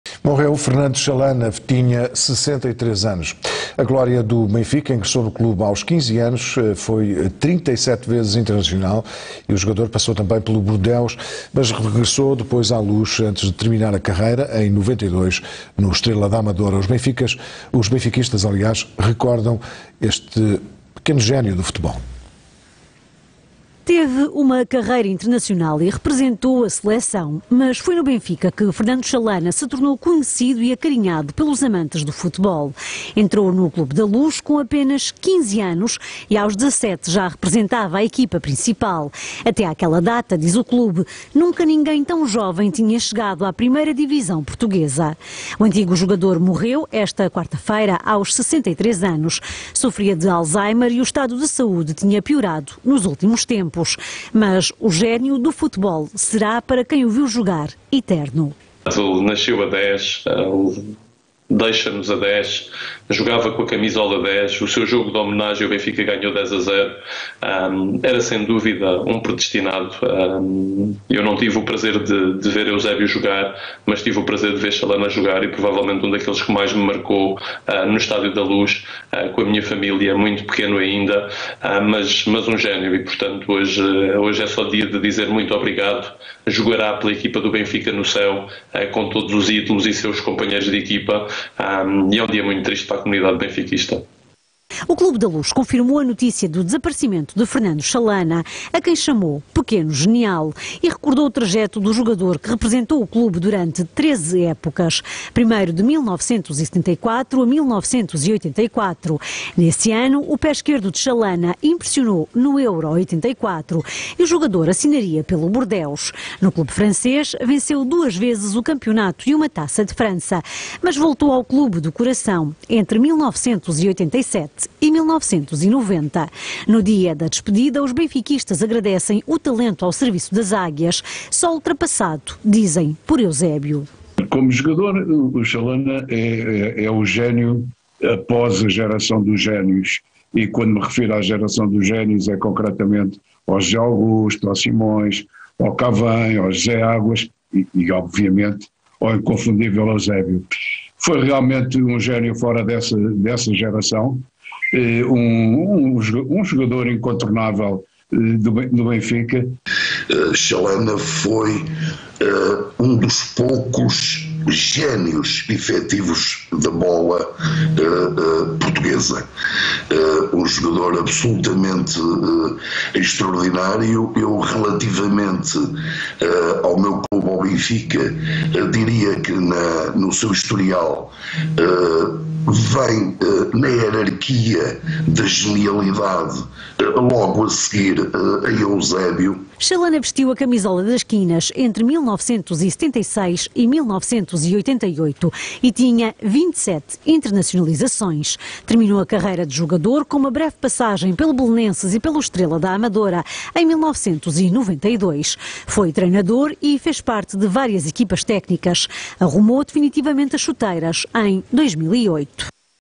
Morreu Fernando Chalana
'O Pequeno Genial' faleceu aos 63 anos.